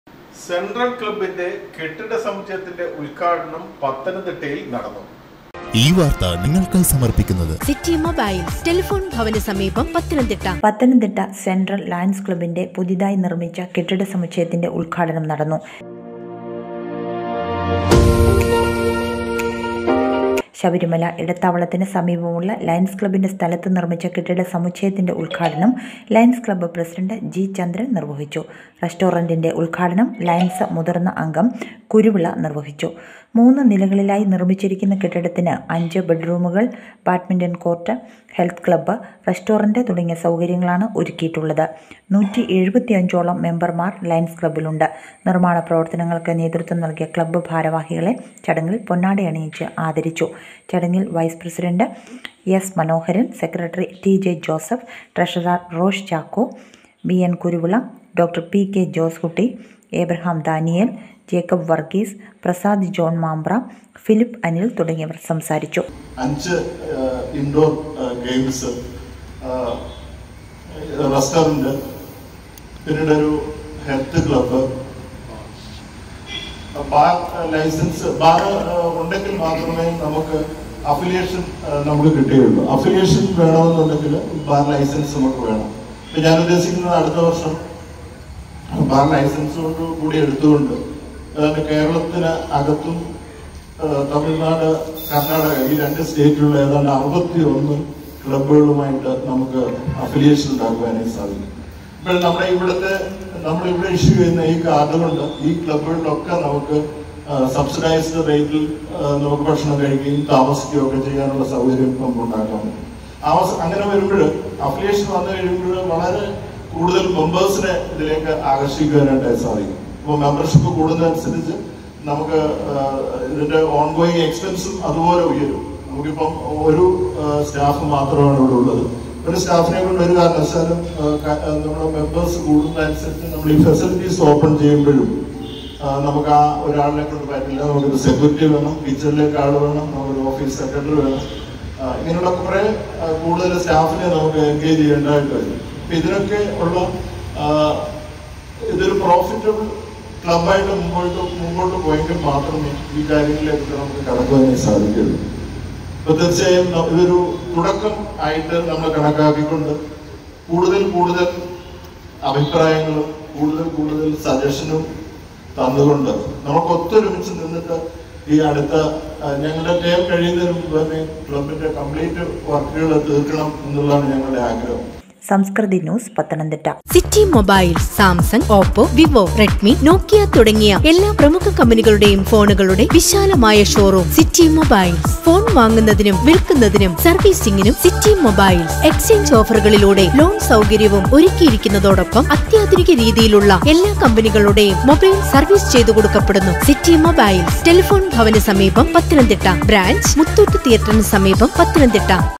പത്തനംതിട്ട സെൻട്രൽ ക്ലബ്ബിൻറെ കെട്ടിട സമുച്ചയത്തിൻറെ ഉദ്ഘാടനം
പത്തനംതിട്ട സെൻട്രൽ ക്ലബ്ബിൻറെ കെട്ടിട സമുച്ചയത്തിൻറെ ഉദ്ഘാടനം  ലയൺസ് ക്ലബ് പ്രസിഡണ്ട് ജി .ചന്ദ്രൻ നിർവഹിച്ചു  !!!